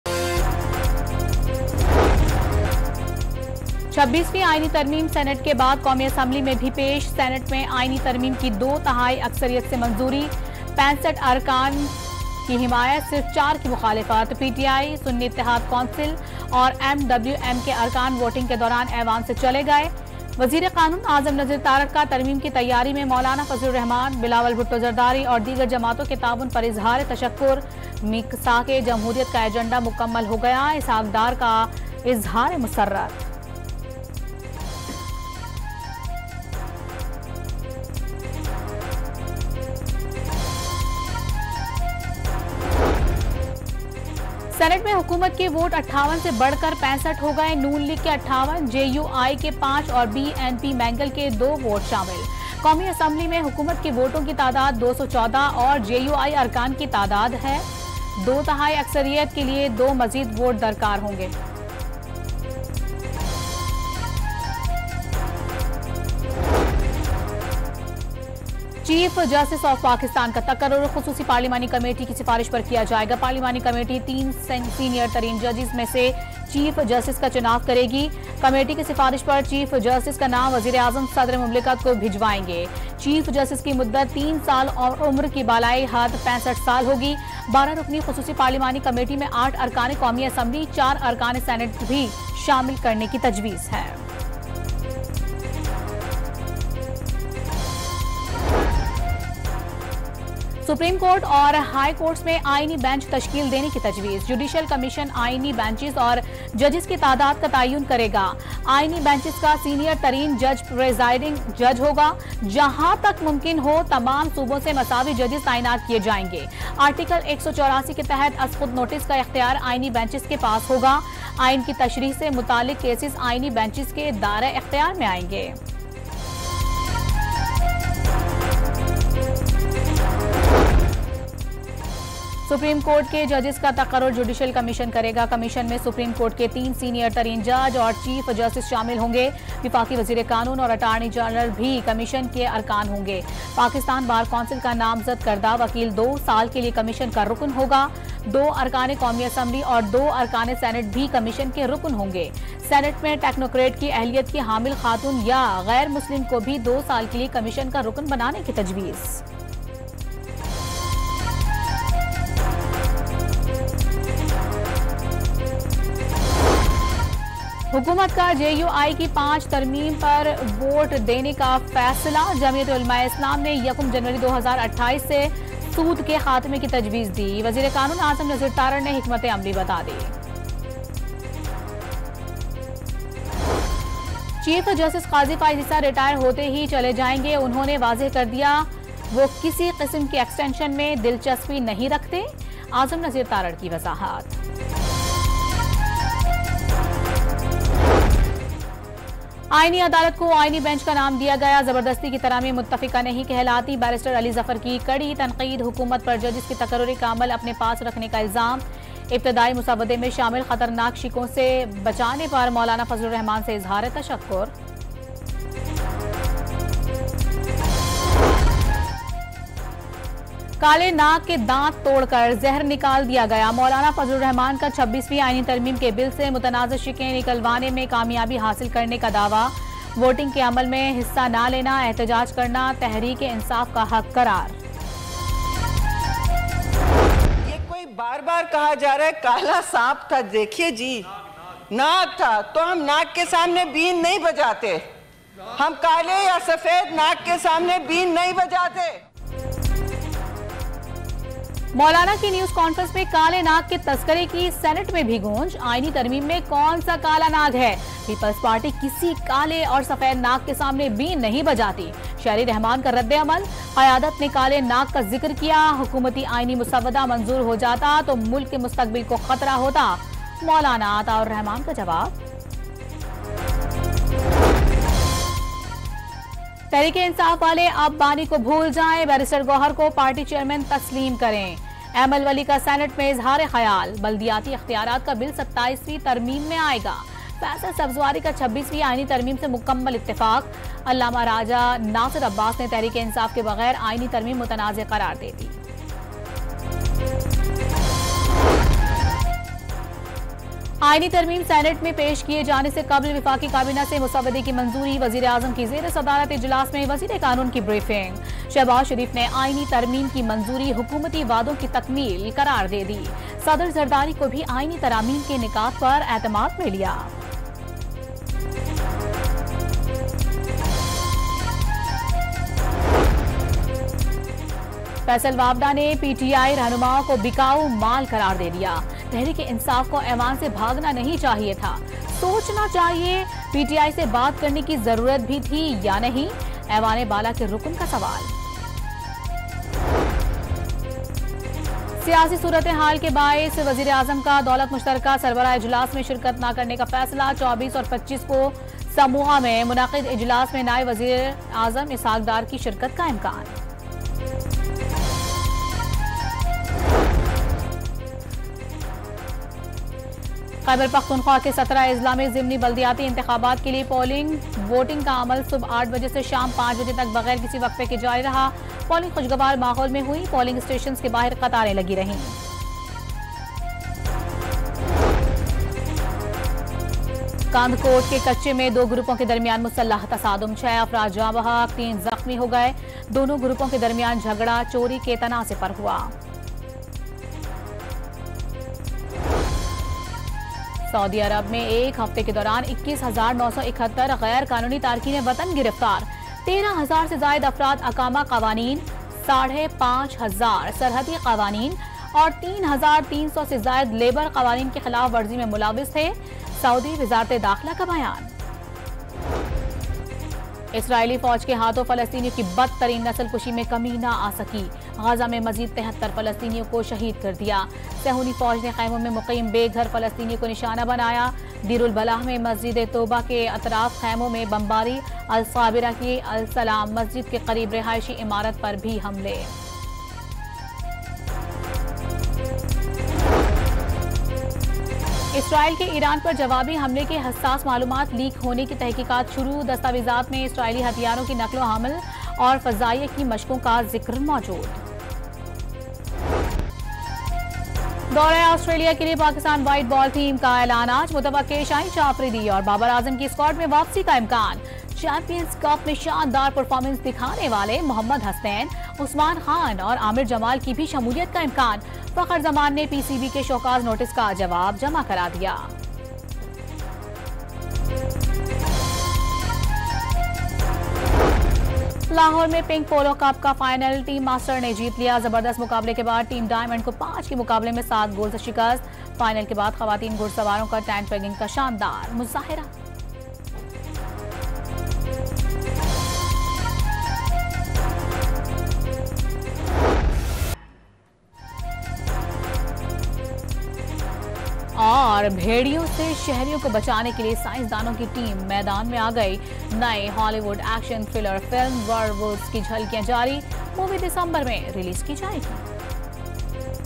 Dawn News Headlines 12 AM | National Assembly Opens Midnight to Pass 26th Constitutional Amendment
Dawn News Headlines : 12 AM | National Assembly Takes Historic Step: 26th Constitutional Amendment

26ویں آئینی ترمیم سینیٹ کے بعد قومی اسمبلی میں پیش کر دی گئی۔